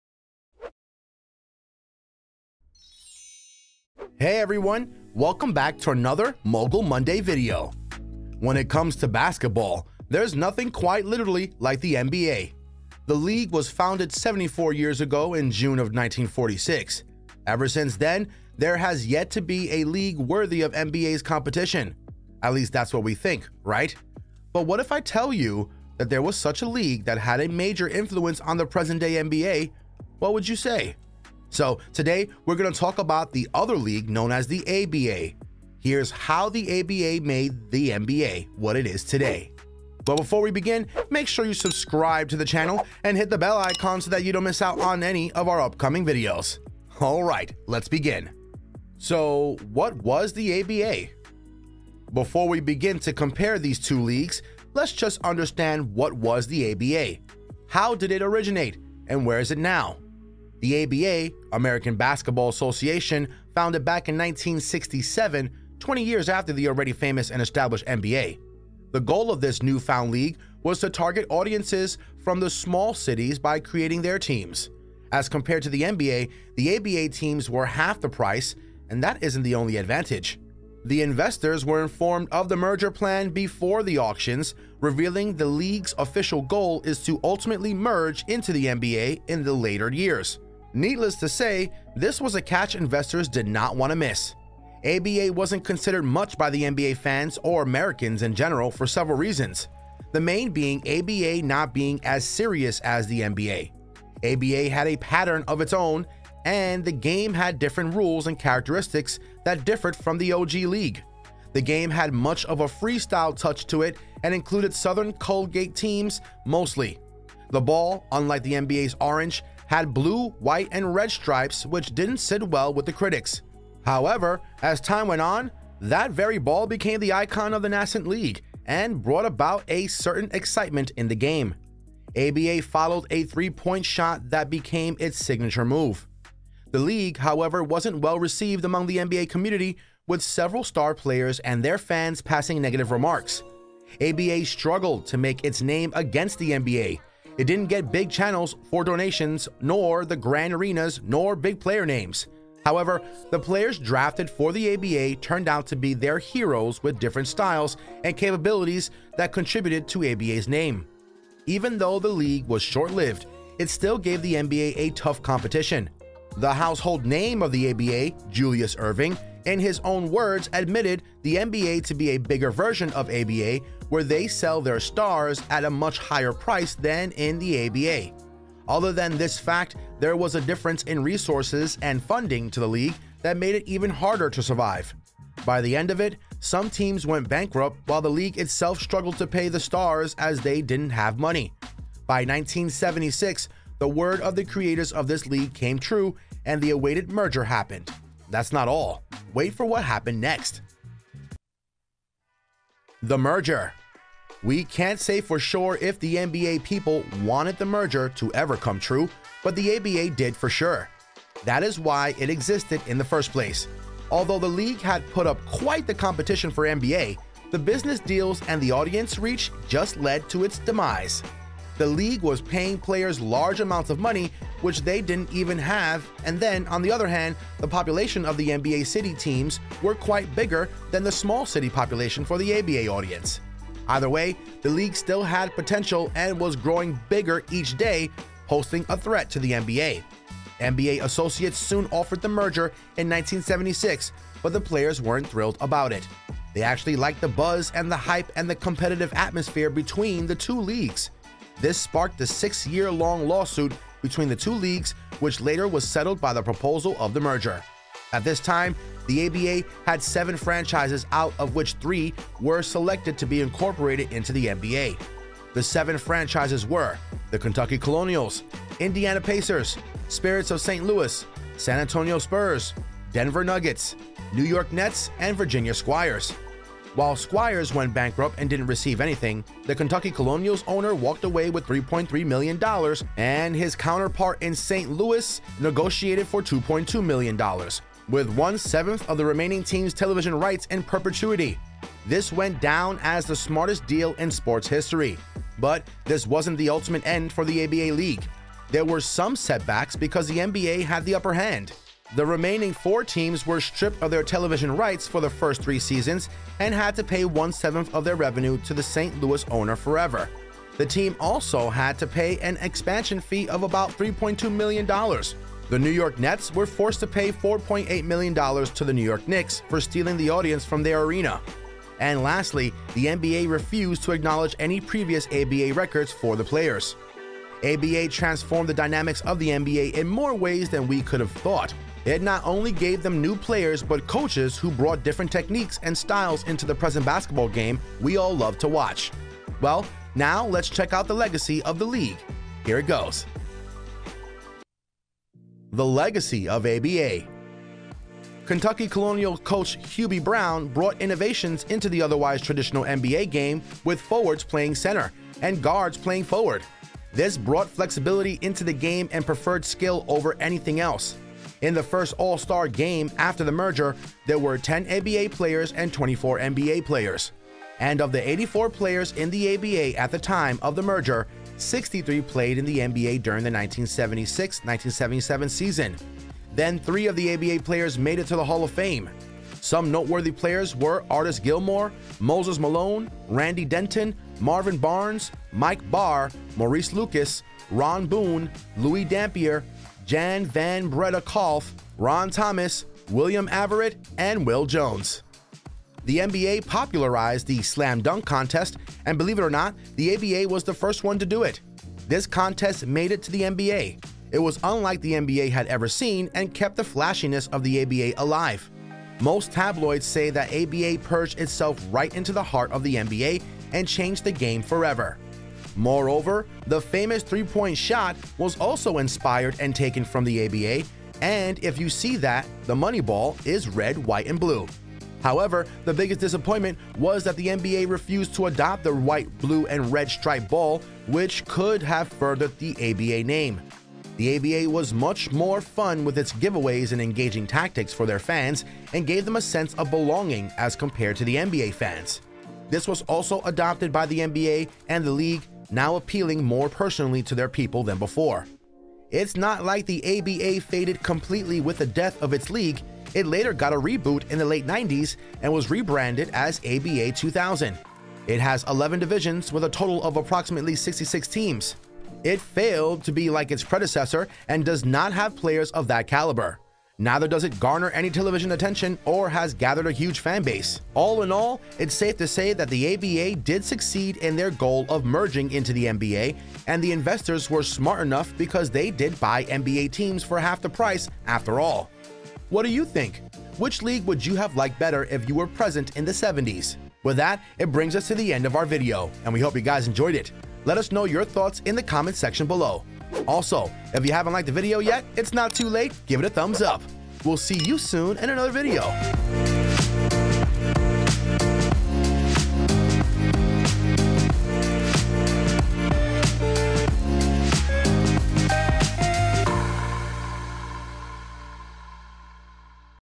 How the ABA Made the NBA What It is Today
Everyone knows what the NBA is, but did you know that there once was a rival league, called the ABA? In this video we take a look at the difference between the NBA and ABA, what happened to the ABA and where they are today. We also take a look at some facets of the NBA today that wouldn't exist without the influence of the ABA. We'd go so far as to say that the ABA made the NBA what it is today.

We are curious about our thoughts? Did the ABA have any influence on the NBA? Do you think our statement is accurate - that the ABA essentially made the NBA what it is today?